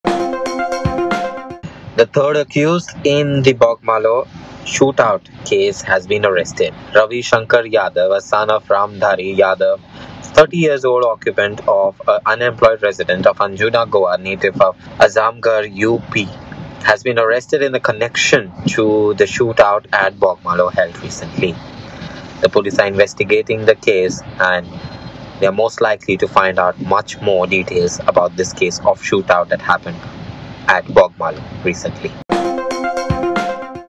Third accused Ravi Shankar Yadav age 30 resident of Anjuna arrested
3rd accused arrested
Ravi Shankar Yadav son of Ram Dhari yadav age 30 occupation unemployed resident of Anjuna Goa native of raghavpur Azamgarh Uttar Pradesh